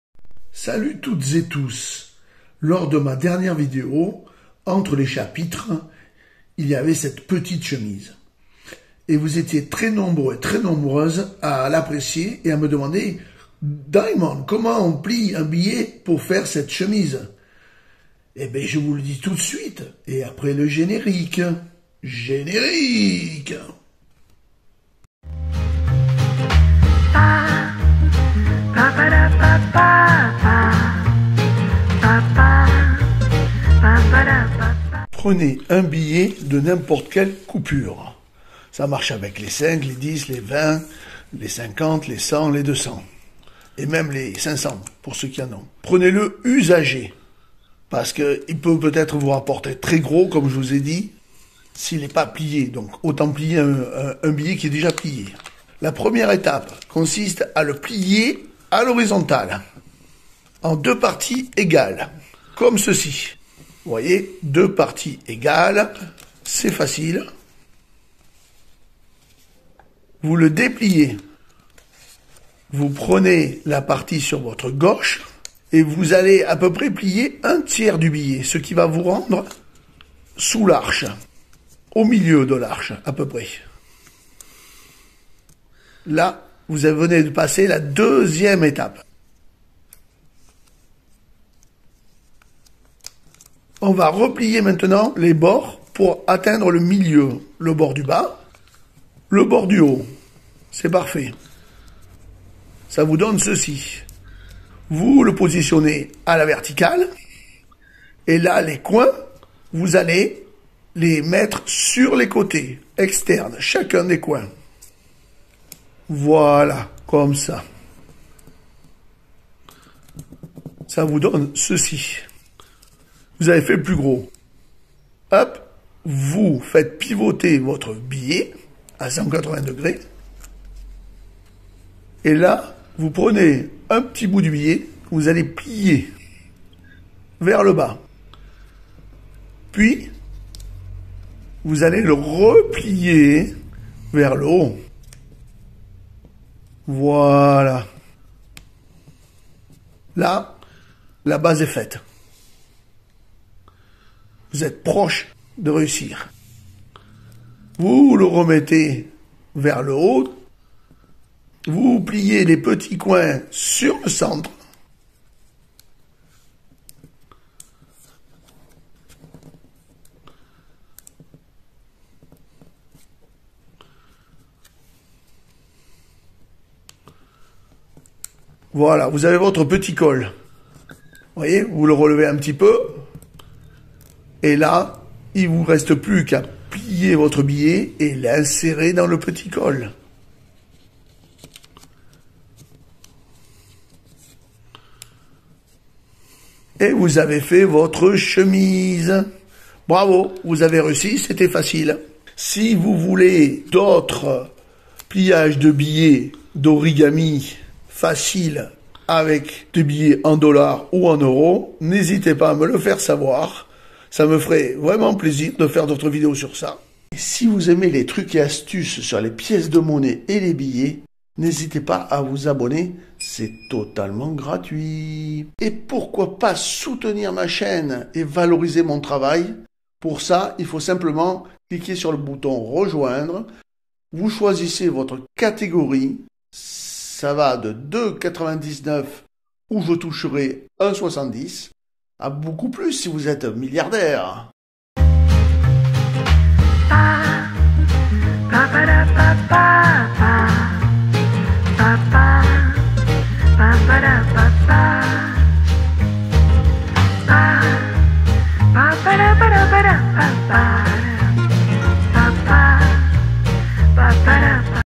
Origami facile débutant - Chemise en billet Dollar Bill Shirt Senbazuru zenbazuru PLIAGE BILLET EURO
Origami SUPER facile débutant - Chemise en billet euro !
🔻🔻🔻 Le Plein d'infos ci-dessous 🔻🔻🔻
LA SEULE VIDEO YOUTUBE D'ORIGAMI COMMENTEE GESTE PAR GESTE PAR L'IMAGE ET LA VOIX , C'EST LE PLUS DE CETTE VIDEO !

vous êtes très nombreux, nombreuses a avoir apprécier une de mes dernière vidéo, entre les chapitres de l'origami facile des pliages de billets en euro pour faire des chemises... et une chemise à 5€, 10€ ou 20€ c'est pas cher !
Origami - Chemise en billet - Dollar Bill Shirt Senbazuru
Un classique parmi les classiques. Si vous ne connaissez pas encore cet origami, voici la chemise que l'on plie traditionnellement dans un billet. Mais vous pourrez très bien la faire avec un rectangle de papier allongé, comme par exemple la chute qu'il vous reste quand vous coupez un carré dans une feuille A4. C'est un modèle populaire dont on ne connaît pas l'auteur.